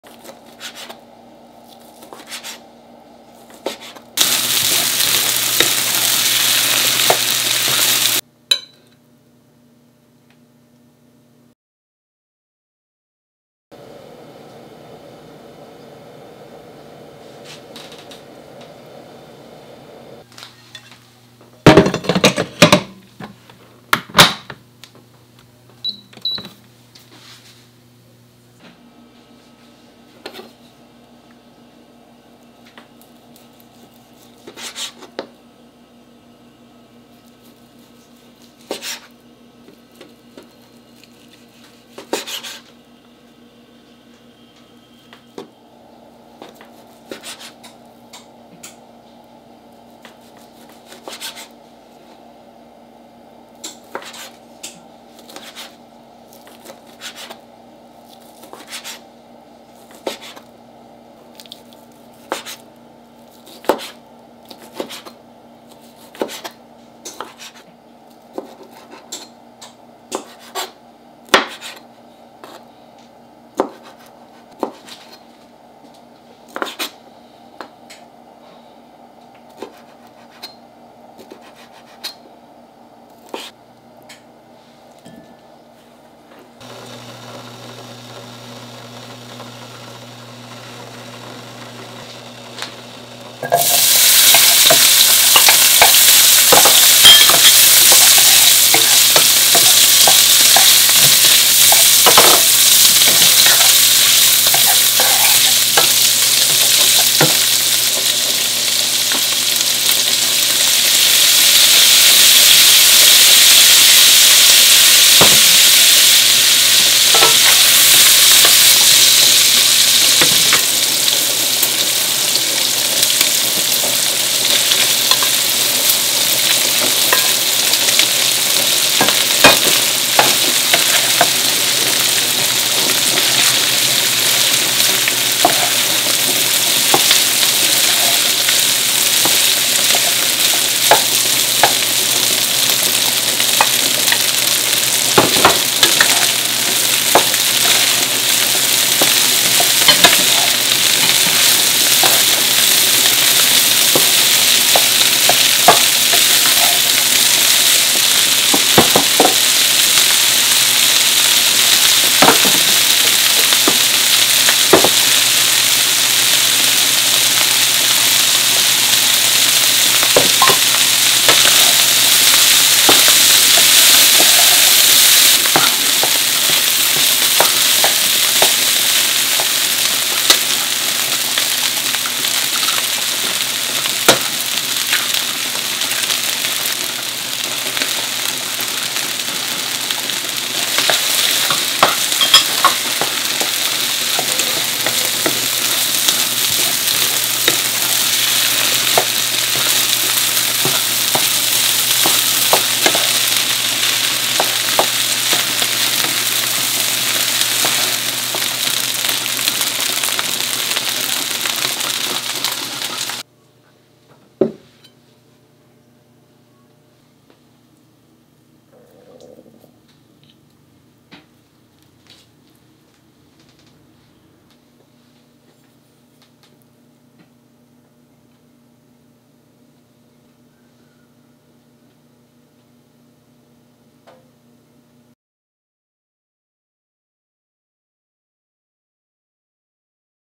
揚州炒飯 (簡易食譜) | Yangzhou Fried Rice or (Easy Recipe) | Riz frit Yangzhou (Recette facile)
☆ 材料/Ingredients ☆

米 Rice   450 g
熟蝦 Cooked Shrimp   12
豉油 Soy Sauce   1 tsp
玉米粒 Corn Kernels   ½ cup
豌豆 Peas   ½ cup
胡椒 Pepper   1/8 tsp
鹽 Salt   1 1/2 tsp
雞蛋 Egg   3

☆ Ingrédients ☆

Riz   450 g
Crevette cuite   12
Sauce soja   1 c. à thé
Maïs en grains entiers   ½ tasse
Pois   ½ tasse
Poivre   1/8 c. à thé
Sel   1 1/2 c. à thé
Oeuf   3

☆ 其他食譜 / Other recipes ☆

☆ Join me ☆